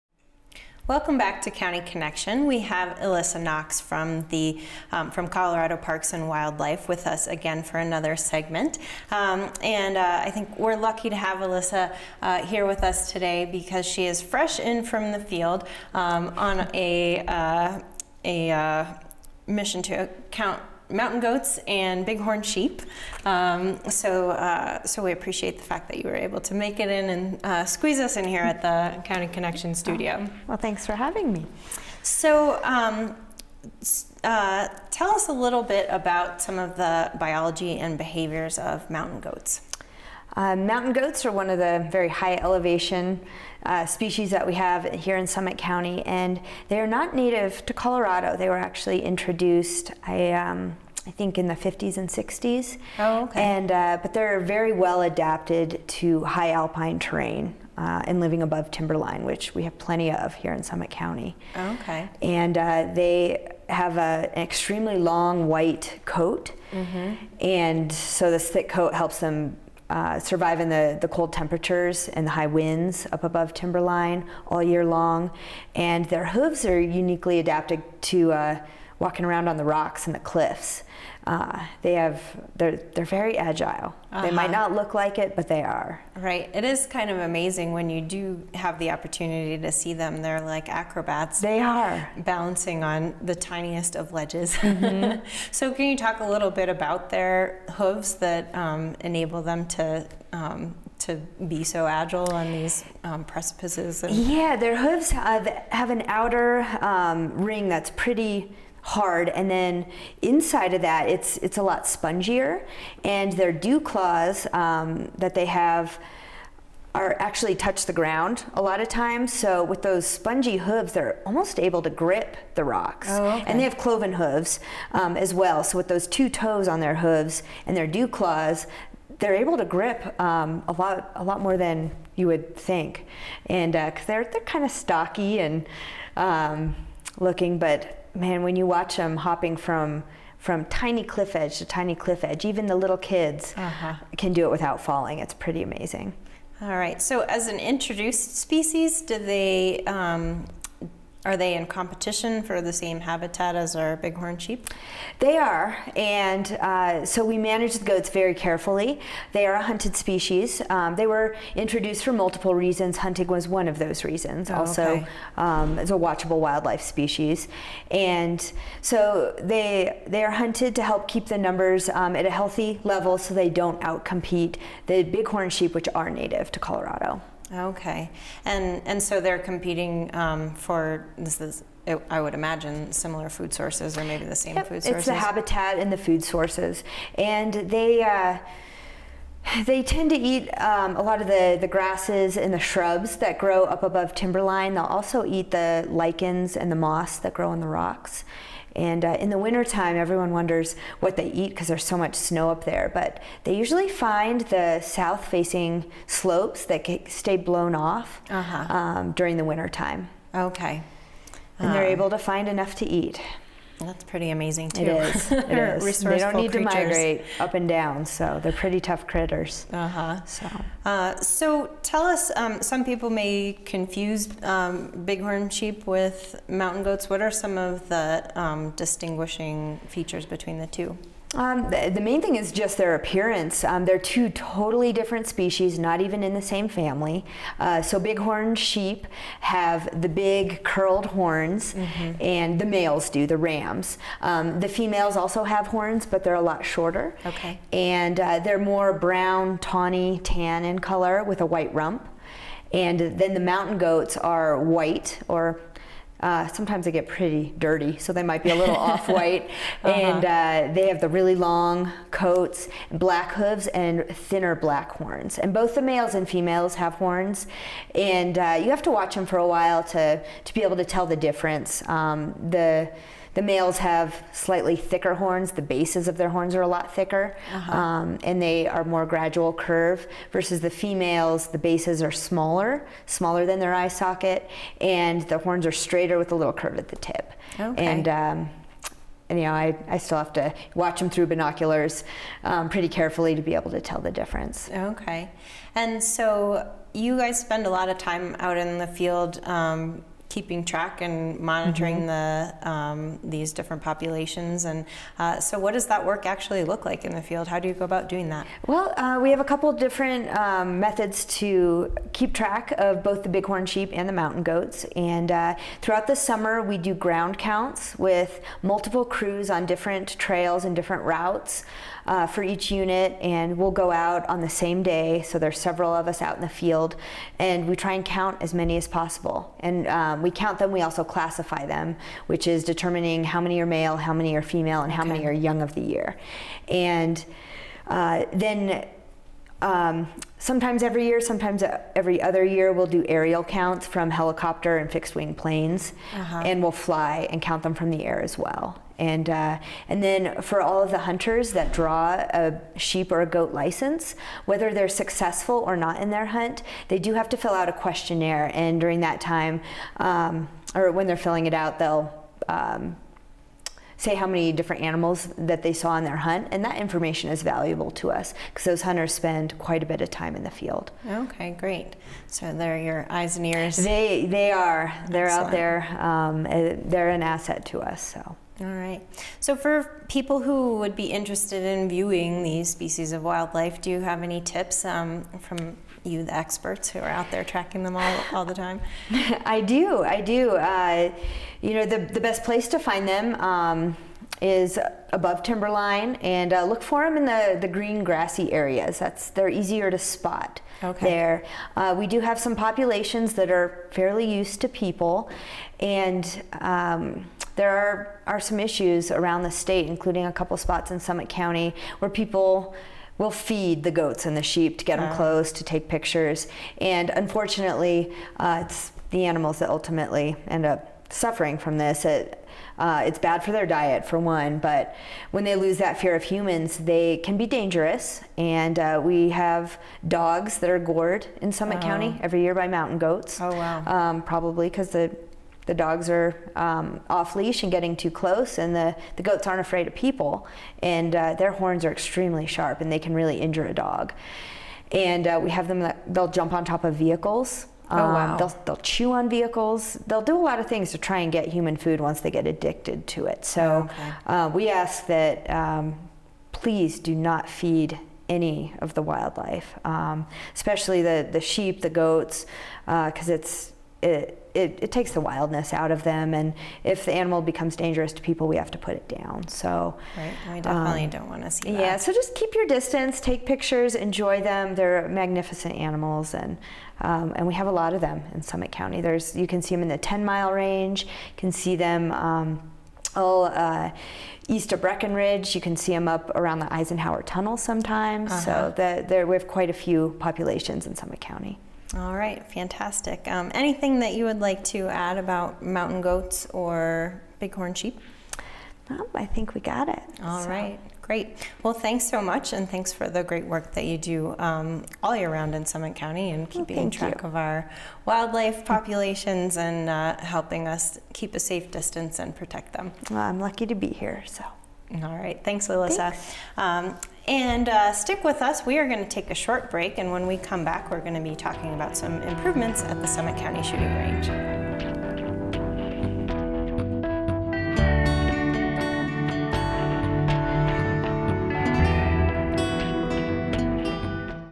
Mountain Goats and Bighorn Sheep in Summit County
Colorado Parks and Wildlife discusses behavior, habitat and biology of bighorn sheep and mountain goats, and provides tips on viewing these fascinating species.